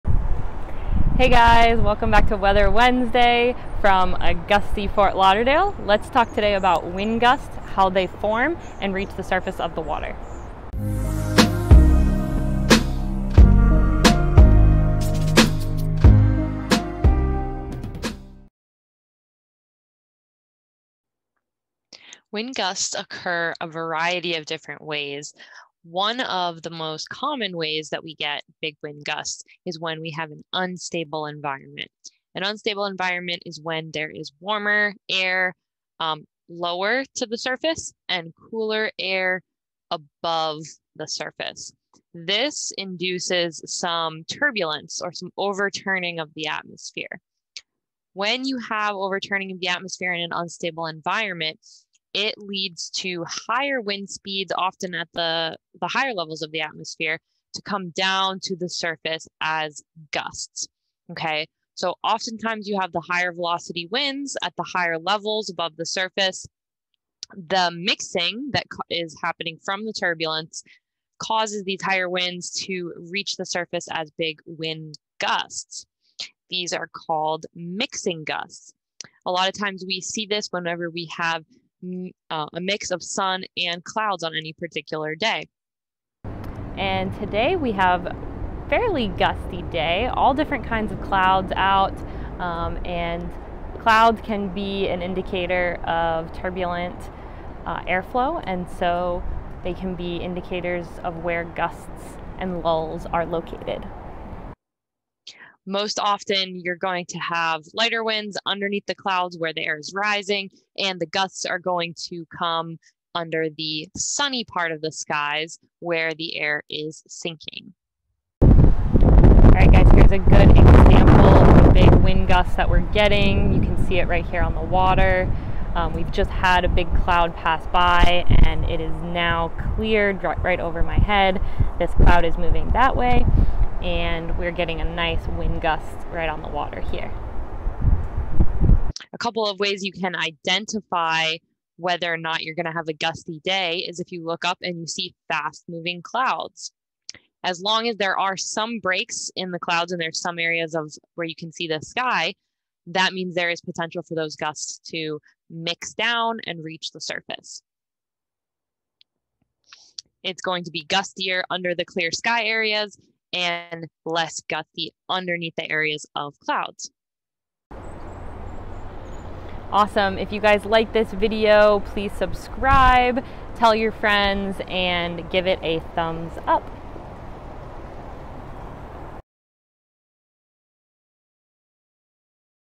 Wind Gusts
This week, join me on a gusty day in Fort Lauderdale to talk all about wind gusts, what causes them, and how they reach the surface of the water. I also give a couple of practical clues on looking for gusts (hint: it involves clouds!)